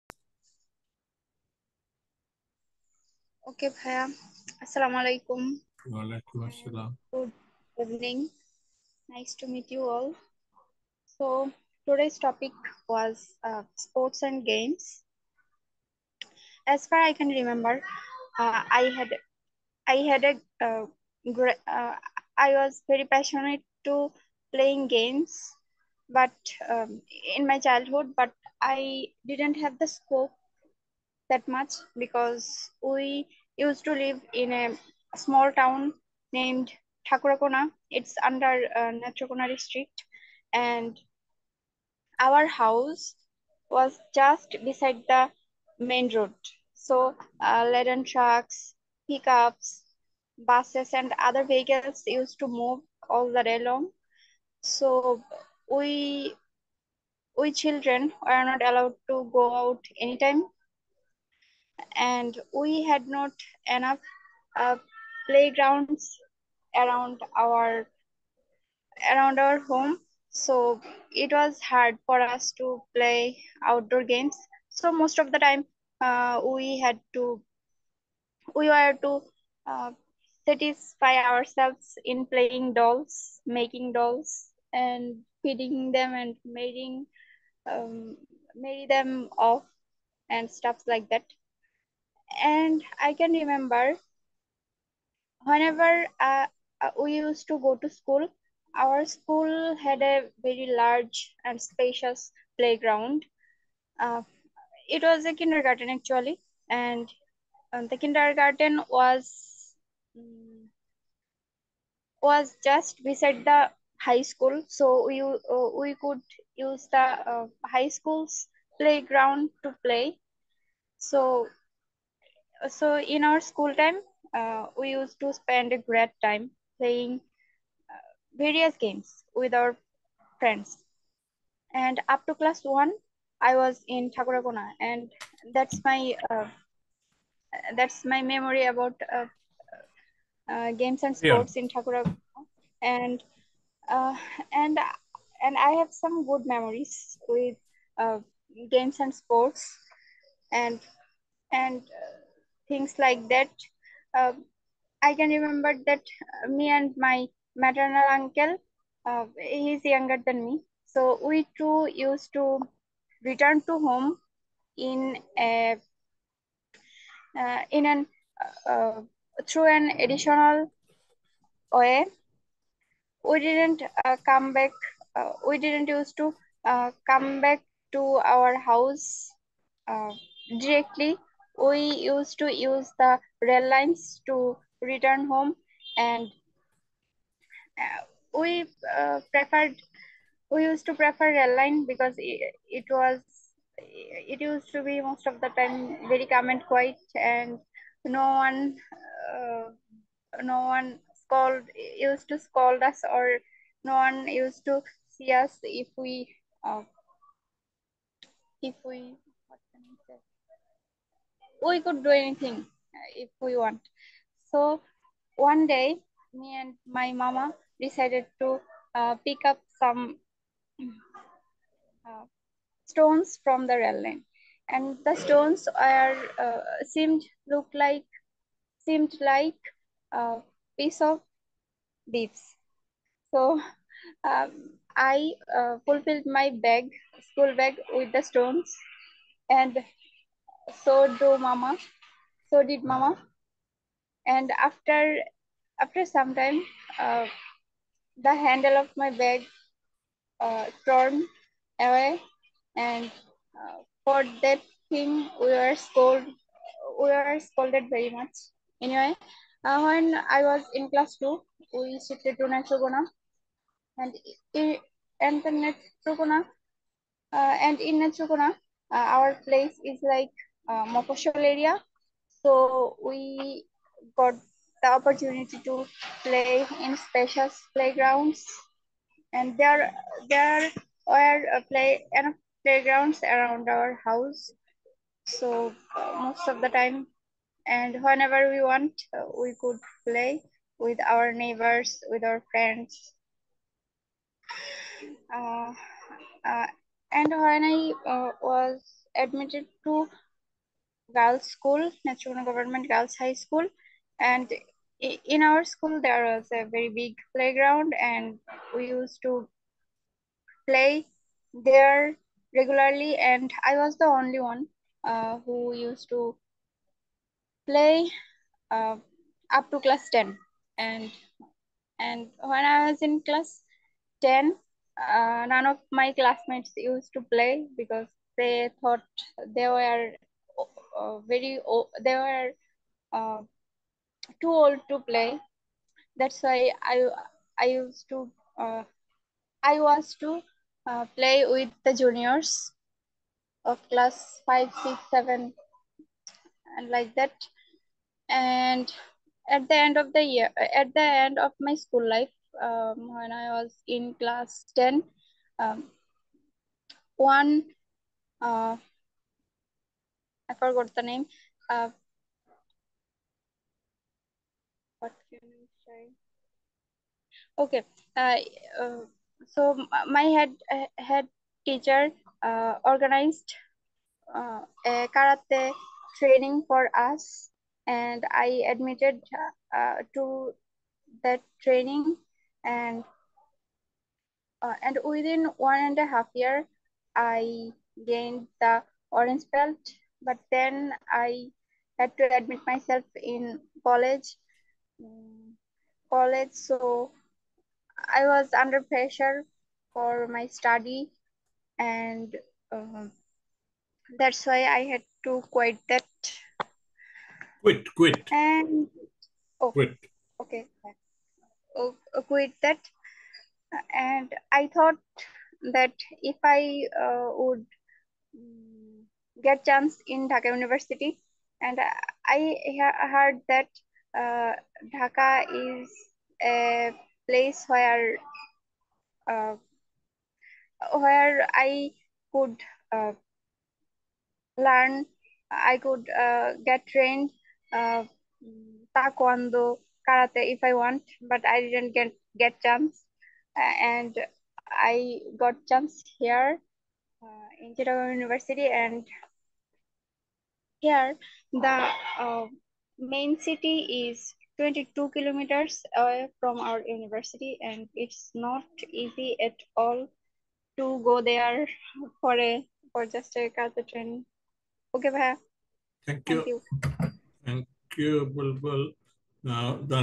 Adda in English 2 - Topic Games - 14/5/2024
Bulbul if Netrokona, a student of Economics at Cittagong University talking about Games